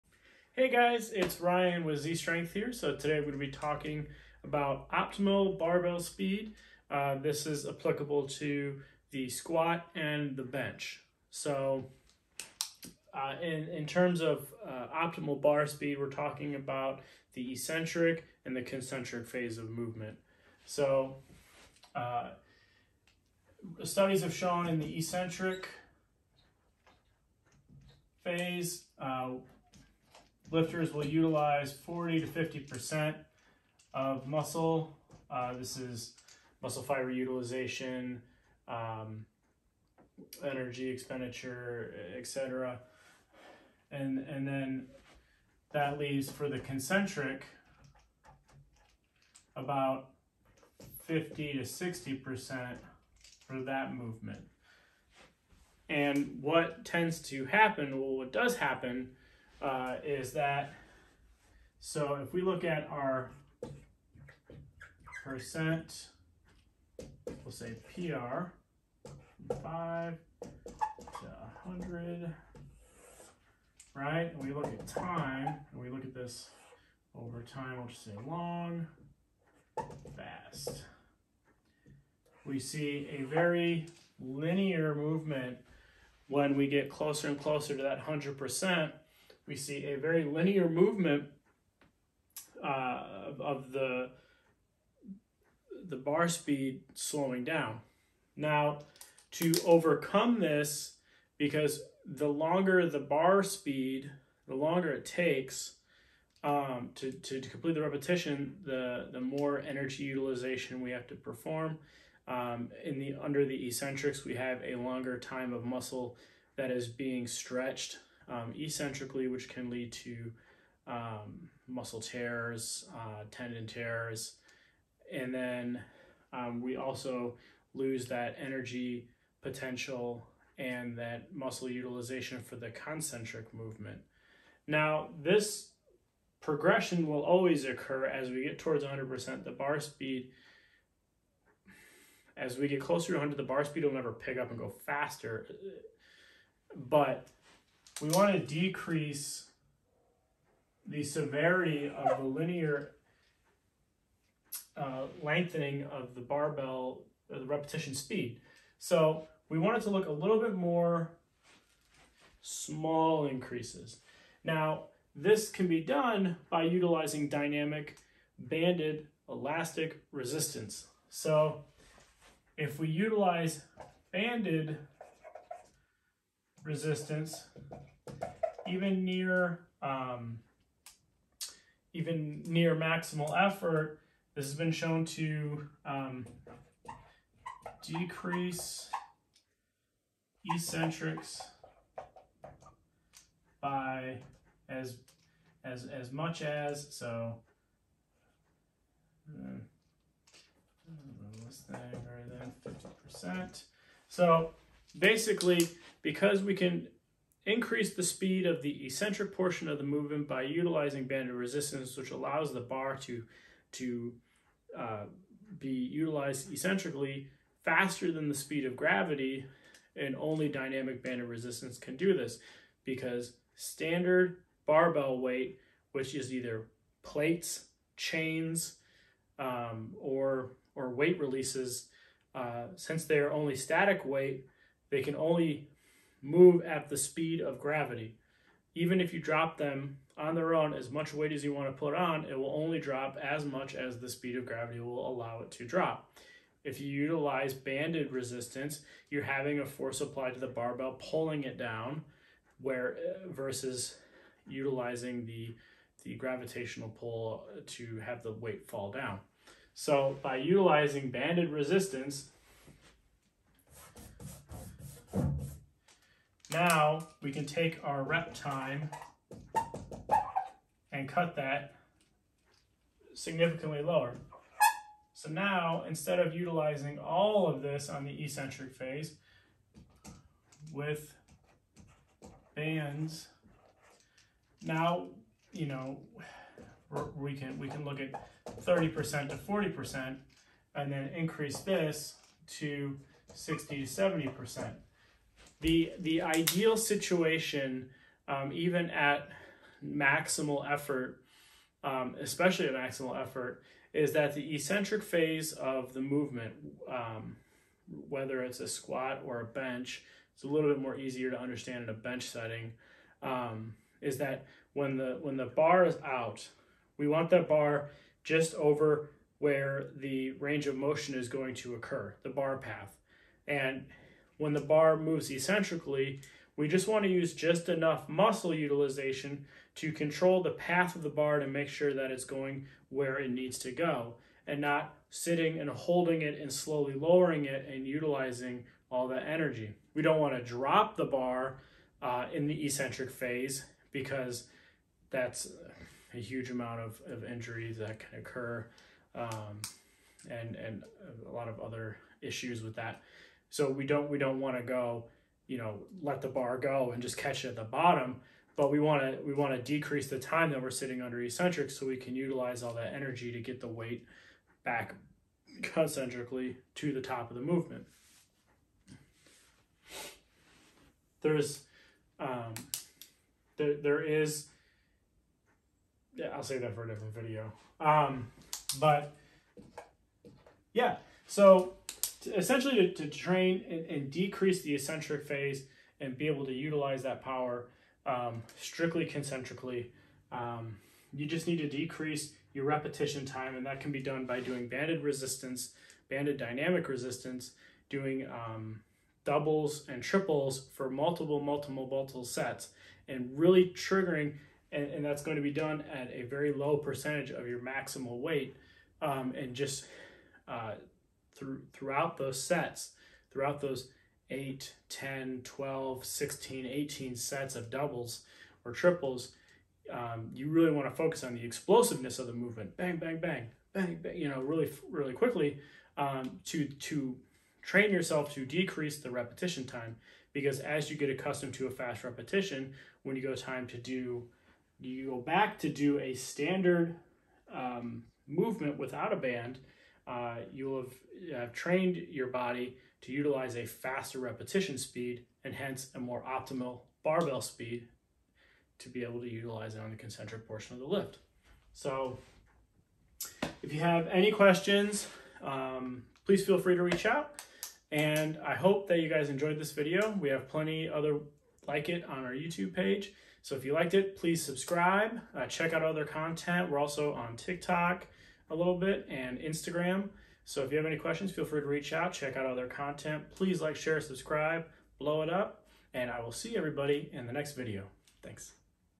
Optimal Bar Speed
Hey everyone! In this video I discuss how to find and gain optimal barbell speed for your bench and squat. In the video I discuss eccentric and concentric movements as well as utilizing dynamic banded resistance to help decrease barbell repetition time.